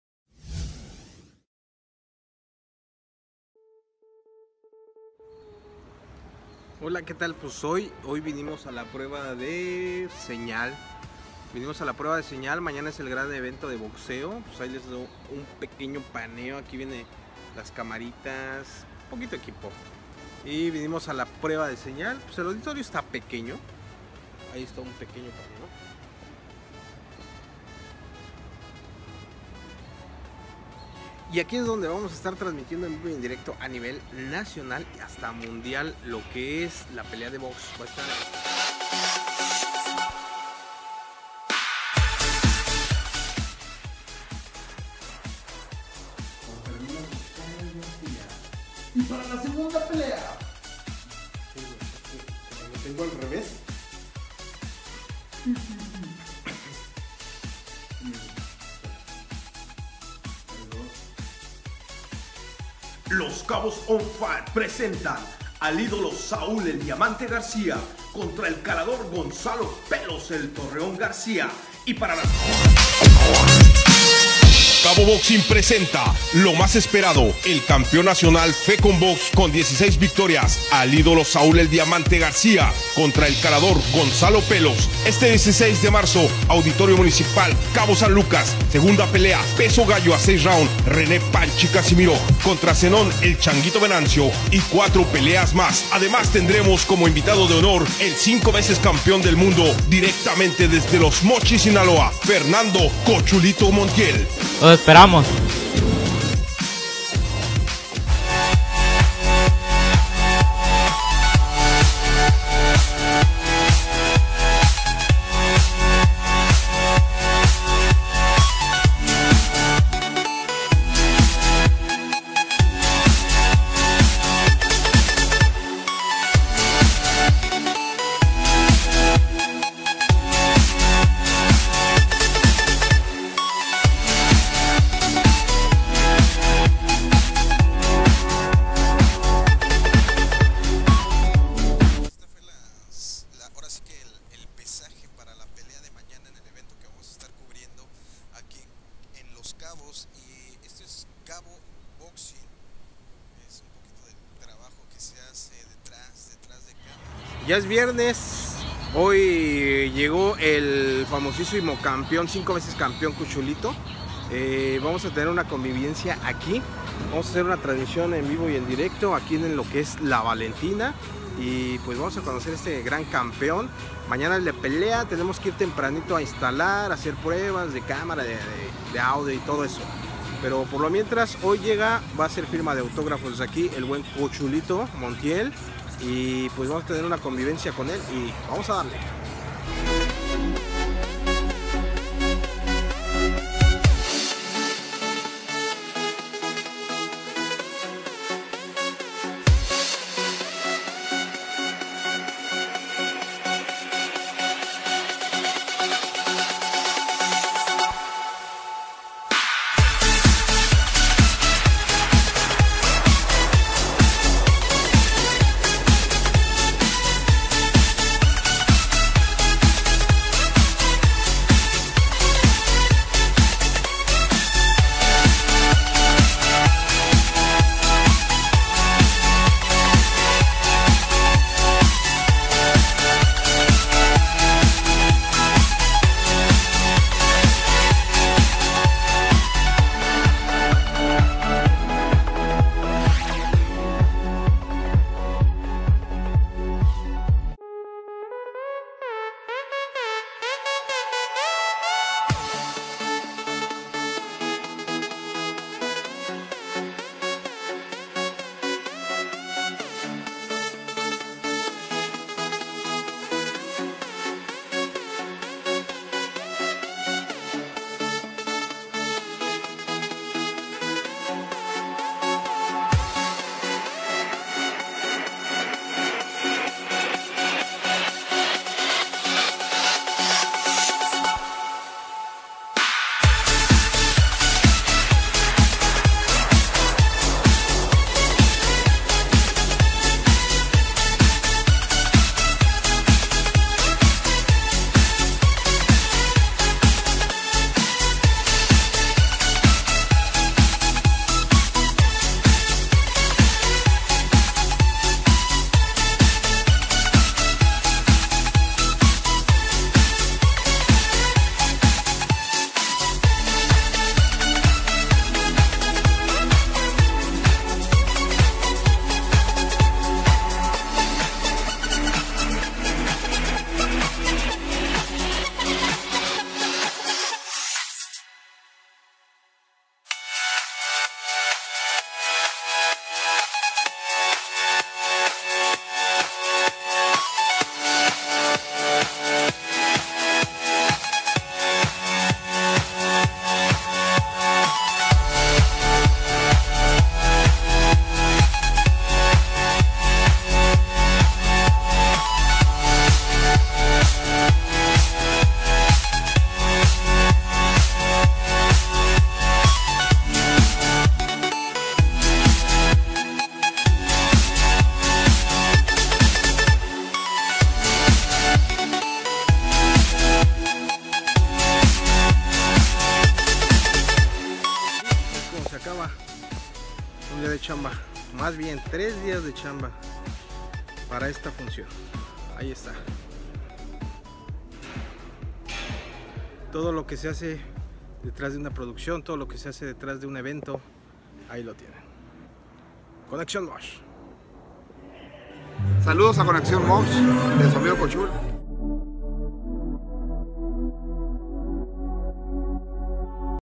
DETRAS DE CAMARAS| Mosh Mora Productions
Todo lo que se hace detrás de una pequeña transmisión